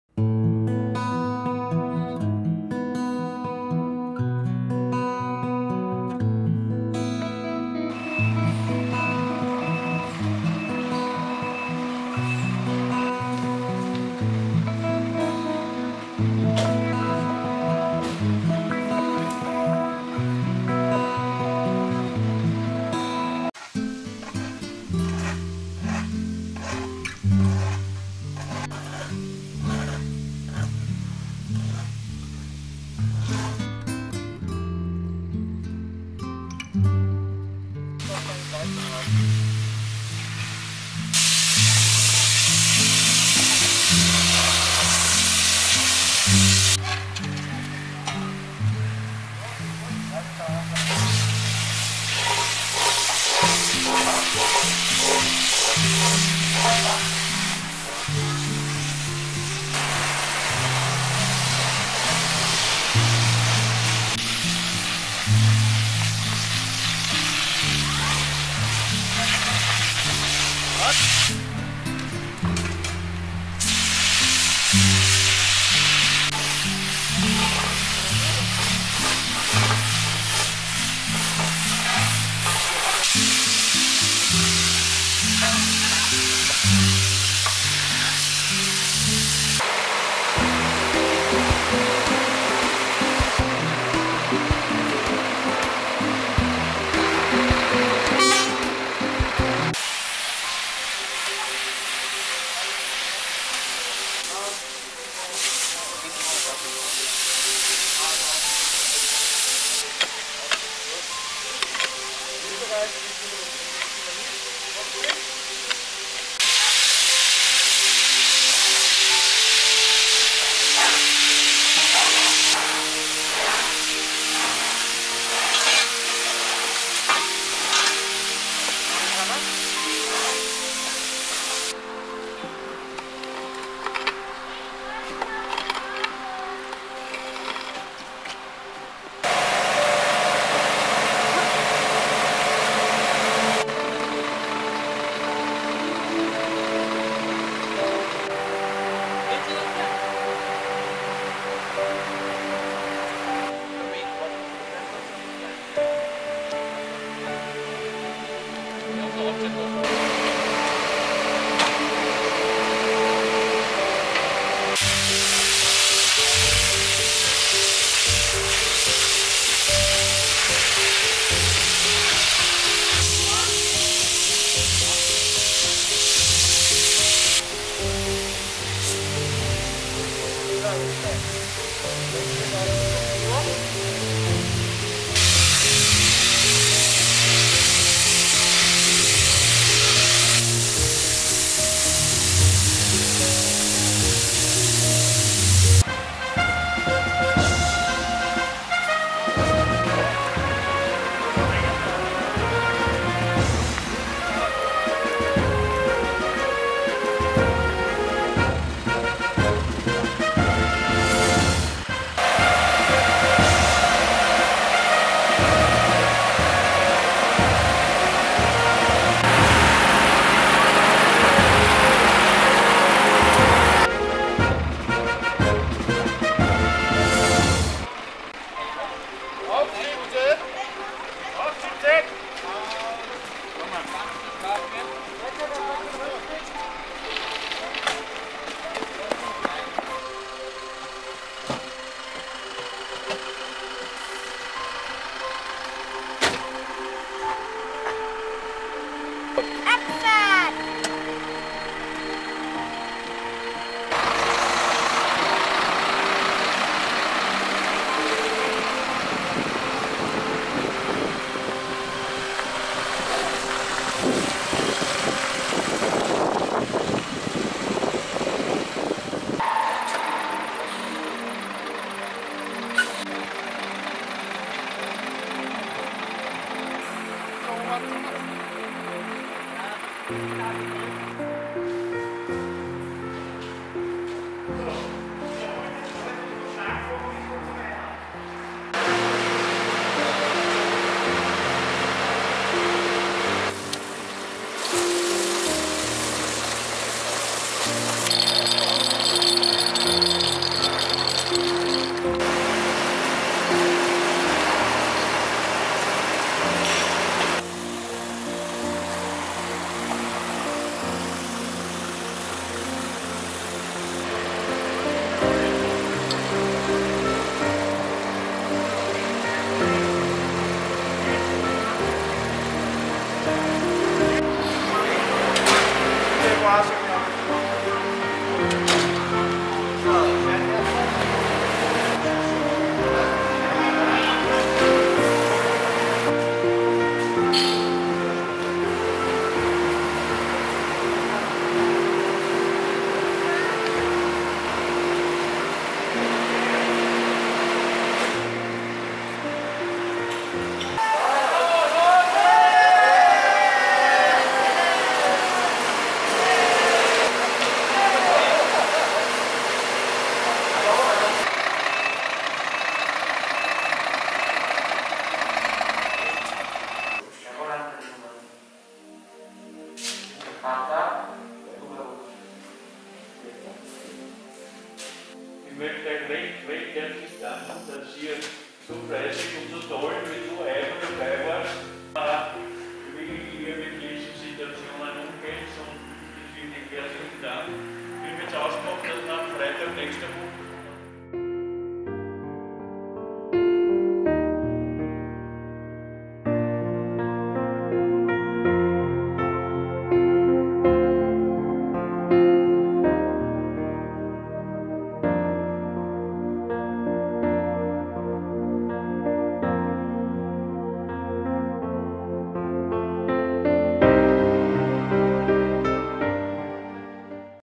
Übung Feuerwehrjugend Klosterfriedhof
Externes Video aus dem Jahre 2020.

Die Feuerwehrjugend Unter-Oberndorf hat im Zuge einer Übung den Klosterfriedhof am Stein (Kat. Gde Unter-Oberndorf) gereinigt.